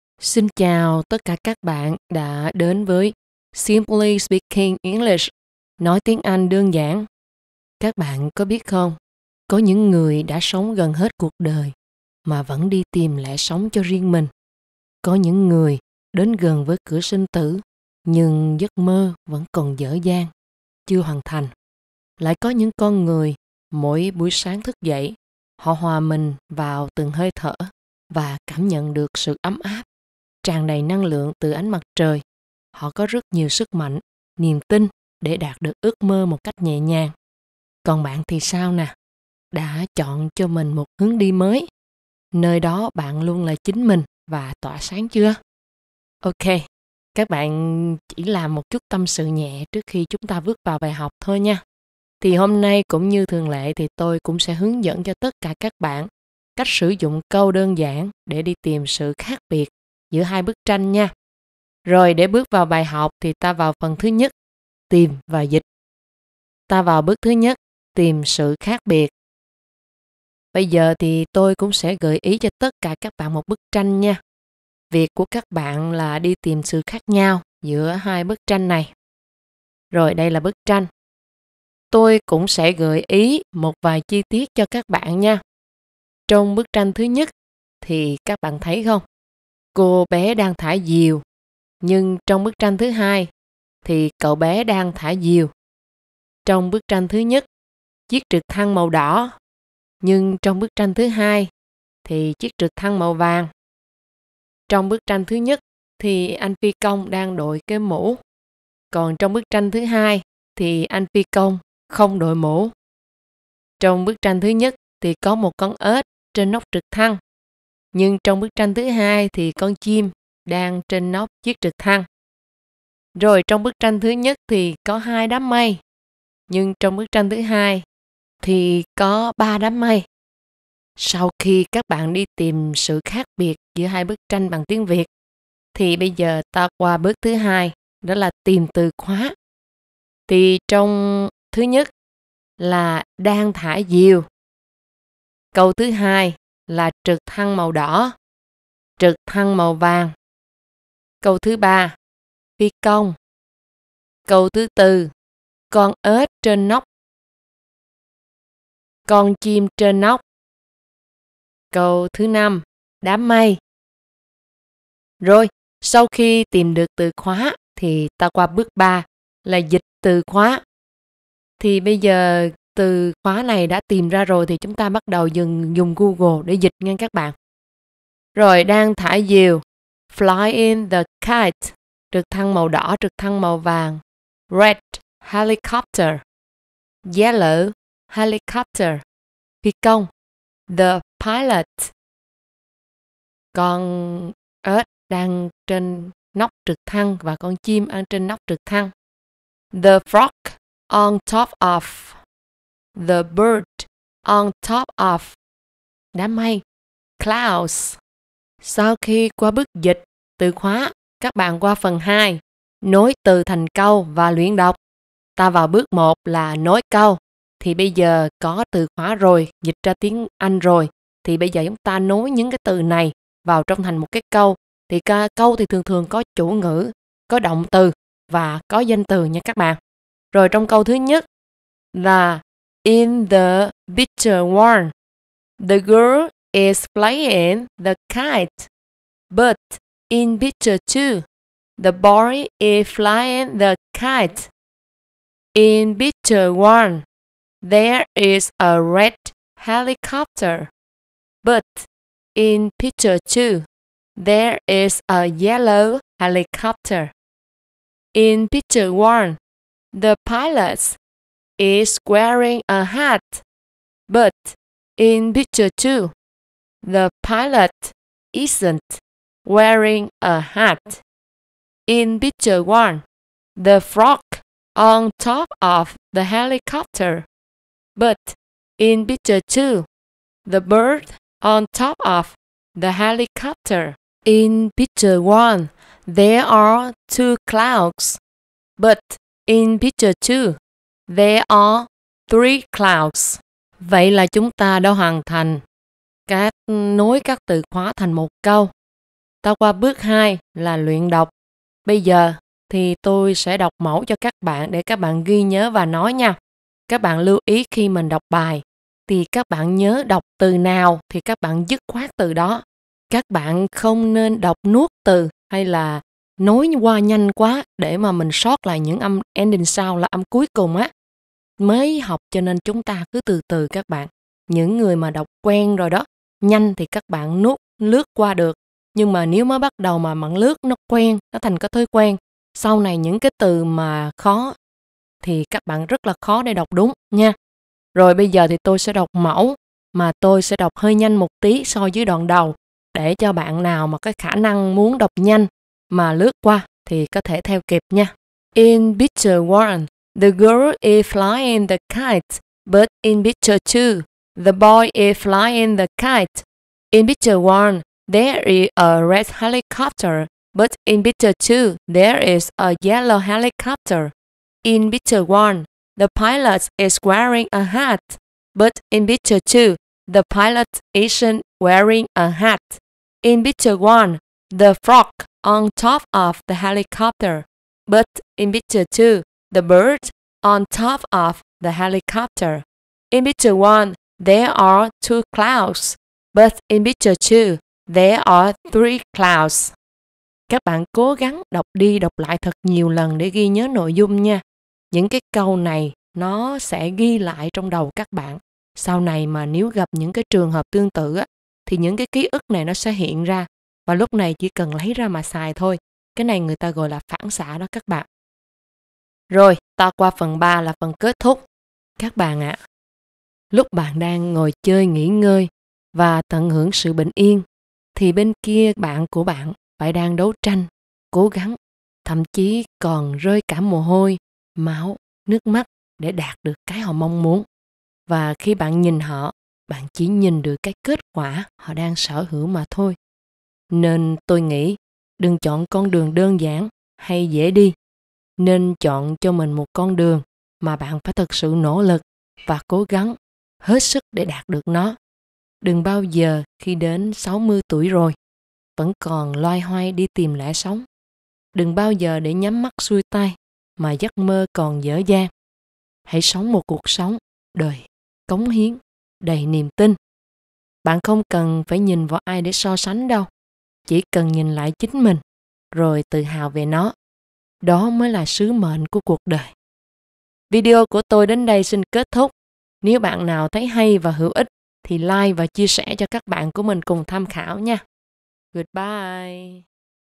Bạn nghĩ nói Tiếng Anh khó đúng không? Không đâu nha! Thật ra rất dễ. Xem thử đi!
Xin chào tất cả các bạn đã đến với Simply Speaking English. Nói Tiếng Anh đơn giản. Các bạn biết không?
Có những người đã sống gần hết cuộc đời vẫn đi tìm lẽ sống riêng cho mình
- Có những con người đến gần với cửa sinh tử
nhưng giấc mơ vẫn còn dở dang chưa hoàn thành.
- Lại có những con người mỗi buổi sáng thức dậy ,
 họ hòa mình vòa hơi thở và cảm nhận được sự ấm áp và tràng đầy năng lượng từ ánh mặt trời. Họ có rất nhiều sức mạnh, niềm tin để đạt được ước mơ một cách nhẹ nhàng.
Còn bạn thì sao? Đã chọn được cho mình một hướng đi nơi đó bạn luôn là chính mình và tỏa sáng chưa?
Ok các bạn chỉ là một chút tâm sự nhẹ trước khi chúng ta bước vào bài học.
Hôm nay tôi cũng sẽ lại hướng dẫn cho tất cả các bạn cách sử dụng các câu đơn giản để tìm sự khác nhau giữa 2 bức tranh nha.
Để vào học ta vào
PHẦN 1 : TÌM VÀ DỊCH
Ta vào bước thứ 1 : Tìm sự khác biệt
Bây giờ tôi sẽ gợi ý cho tất cả các bạn một bức tranh nha.
Việc của các bạn là đi tìm sự khác nhau giữa 2 bức tranh này nha
Tôi cũng sẽ gợi ý một vài chi tiết cho các bạn nha.
Trong bức tranh thứ 1 : Cô bé đang thả diều, nhưng trong bức tranh thứ 2 Cậu bé đang thả diều.
Trong bức tranh thứ 1 : Chiếc trưc thăng màu đỏ, trong bức tranh thứ 2 chiếc trực thăng màu vàng
Trong bức tranh thứ 1 : Phi công đang đội mũ, bức tranh thứ 2 phi công không đội mũ
Trong bức tranh thứ 1 : có 1 con ếch trên nóc chiếc trược thăng, nhưng trong bức tranh thứ 2 thì con chim đang trên nóc chiếc trực thăng
Trong bức tranh thứ 1 có 2 đám mây, trong bức tranh thứ 2 có 3 đám mây
Sau khi tìm được sự khác biệt bằng Tiếng Việt thì ta qua